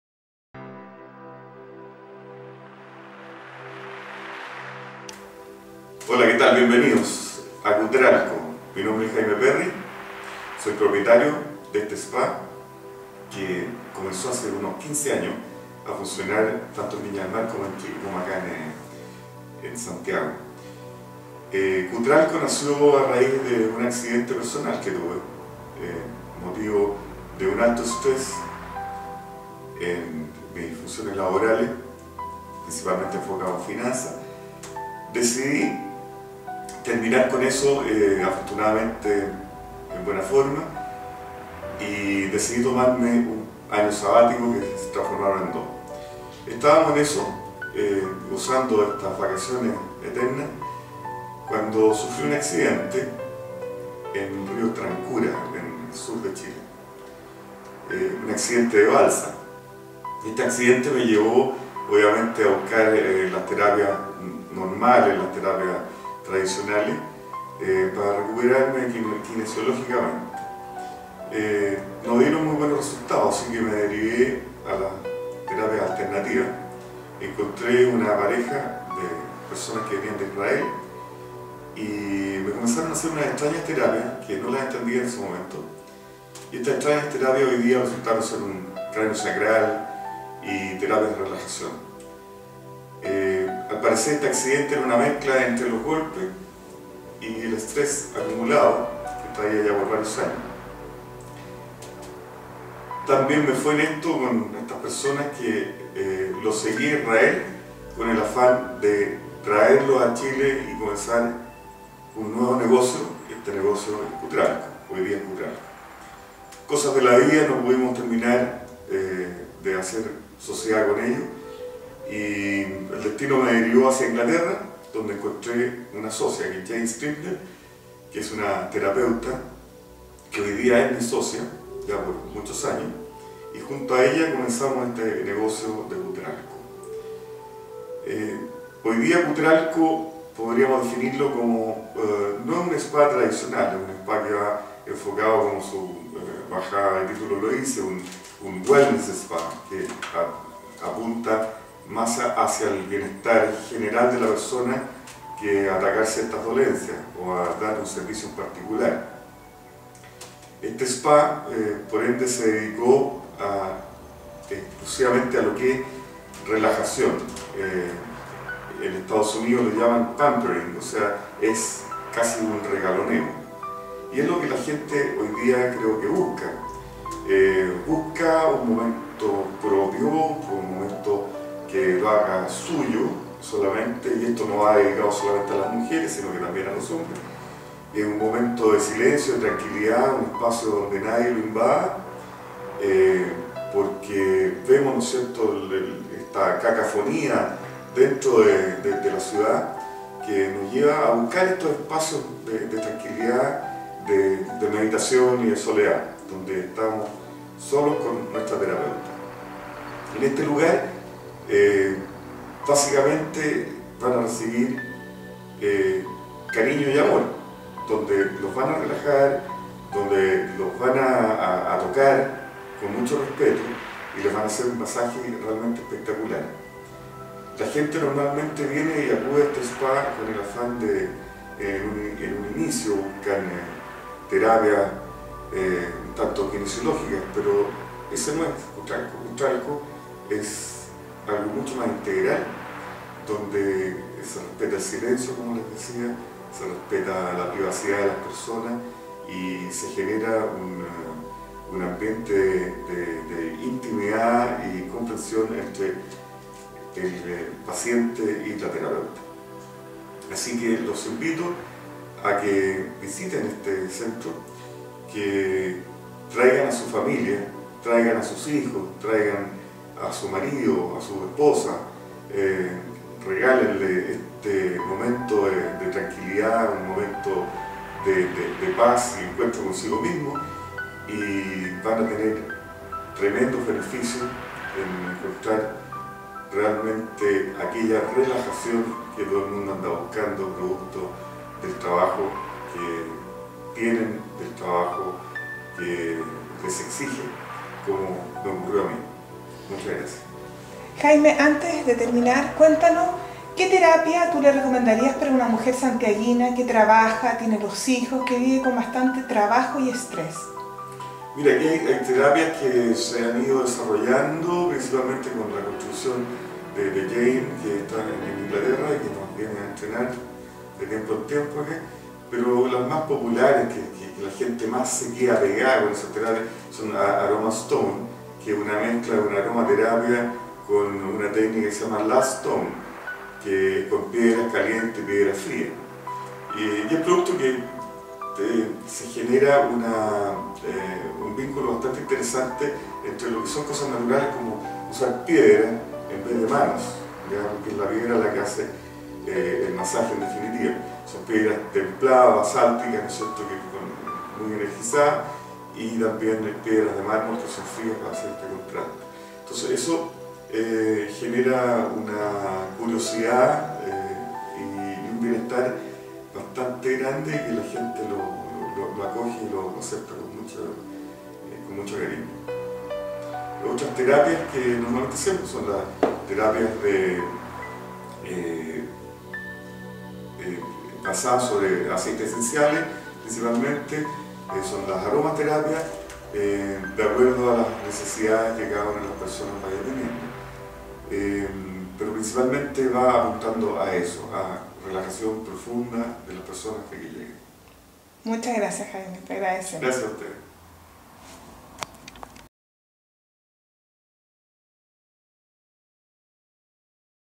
Kutralco, el Wellness Spa de Chile. Conversación con Jaime Perry
Jaime Perry no explica cómo nació Kutralco,  el primer Wellness Spa de Chile y las terapias que ofrece.